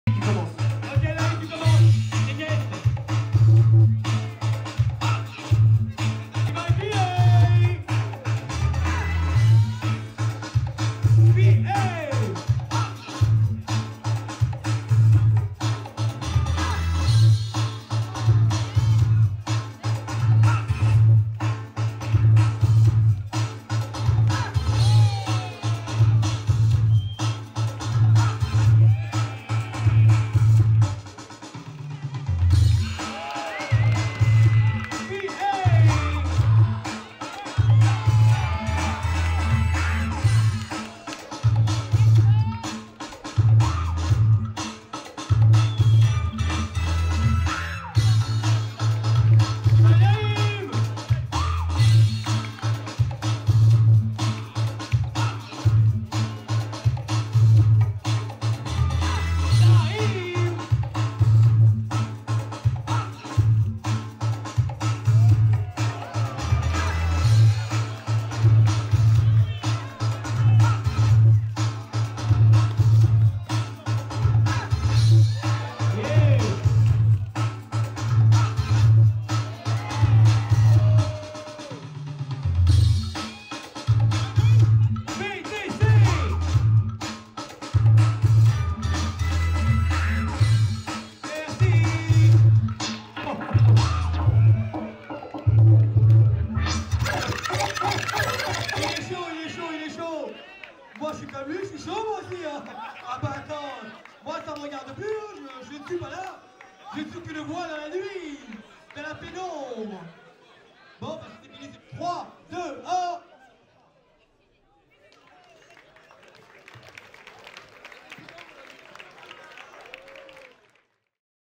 Bboy naim vs bboy P.A / quart de final Break / next urban legend 2022
Next urban legend 2022
Édition 9 (ville de Sevran)

Quart de final break
Bboy naim vs bboy P.A

🎵🎧 dj nep

Thanks all dancers …